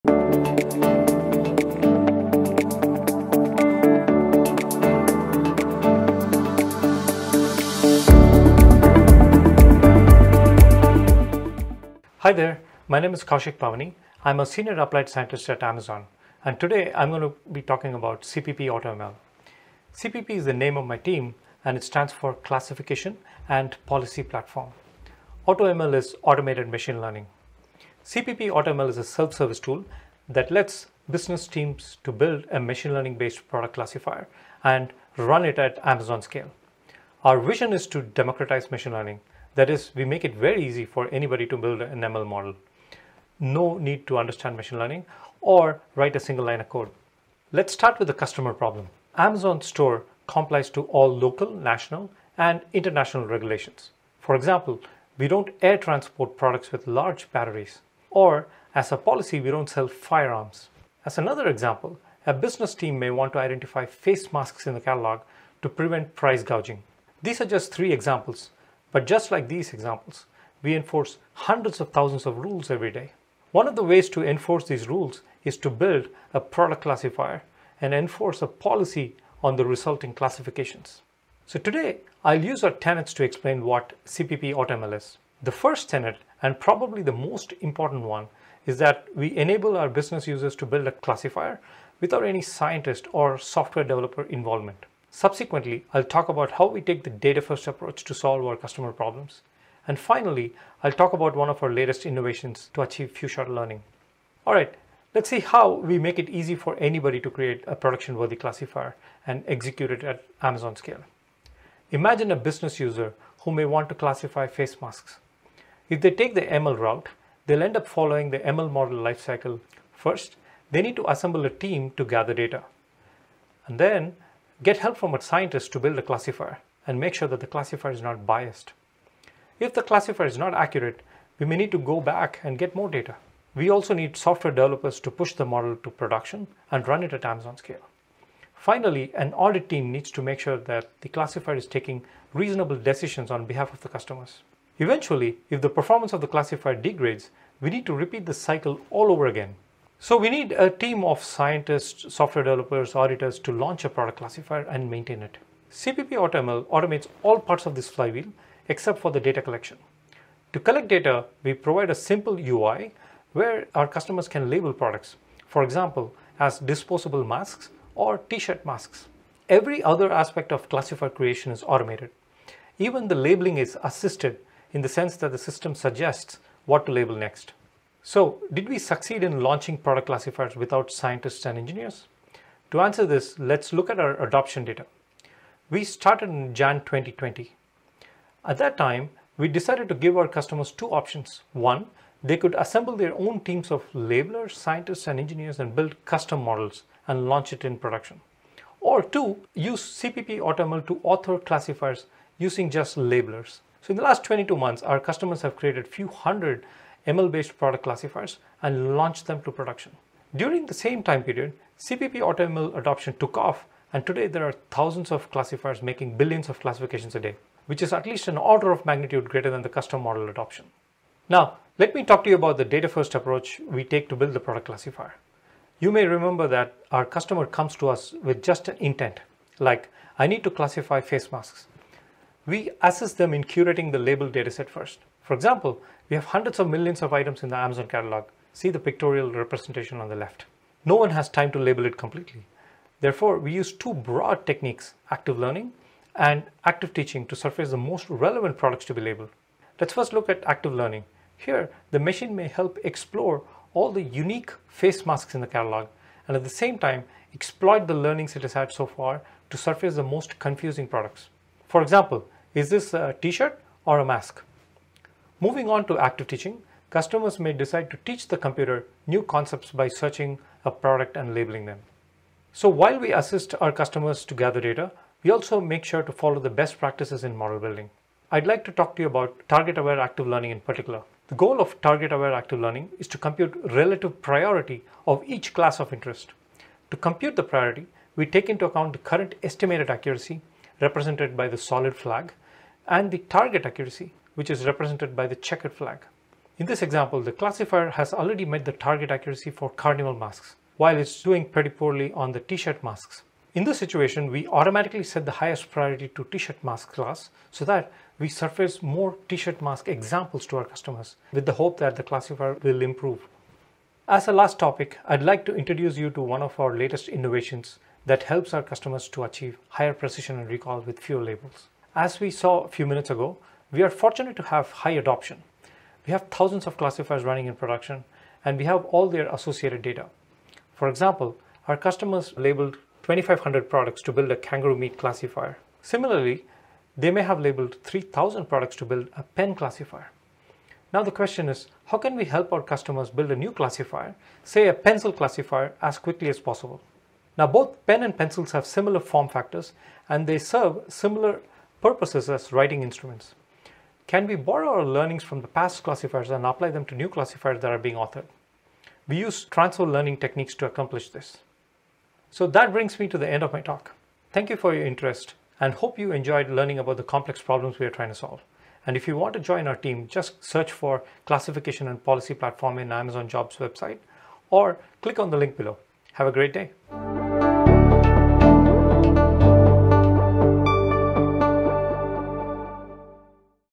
The Story Behind Selections - Safety and Compliance
Ever wonder how we keep Amazon’s selection safe and compliant globally? In this talk, Kaushik Pavani, Sr. Applied Scientist in Classification and Policy Platform (CPP) talks about "CPP AutoML"—an automated self-serve machine learning platform that enables business teams to create custom classification logic and identify non-compliant products. Kaushik describes the key tenets that propelled CPP AutoML's growth, delivering thousands of product classifiers end-to-end without engaging scientists or engineers.